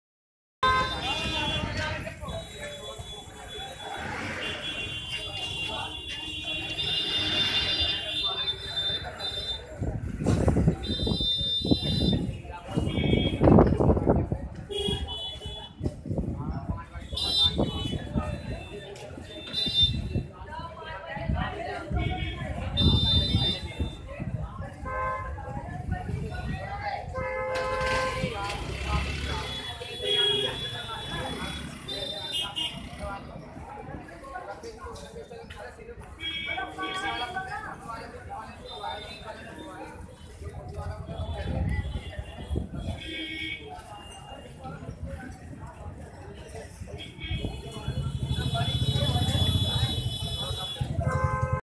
Even after 70 years of Independence.... In Delhi MCD workers doing their job in this way
North DMC Workers doing their job in Delhi Sadar..Bazar. in this way.